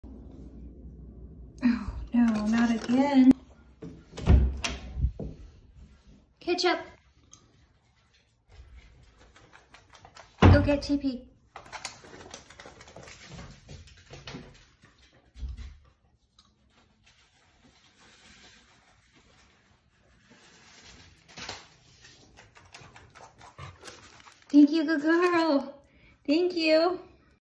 Corgis’s best trick - bring toilet paper
Our corgi Ketchup brings Toilet Paper on demand! She is a really good girl!

She will bring various other household item when asked as well.

At our old house we also taught her to open the fridge so she can fetch beverages. And yes she knows how to close the door too.

Ketchup is our fur baby and we love her very much. She is very task oriented and enjoys having these “jobs”.

Thanks!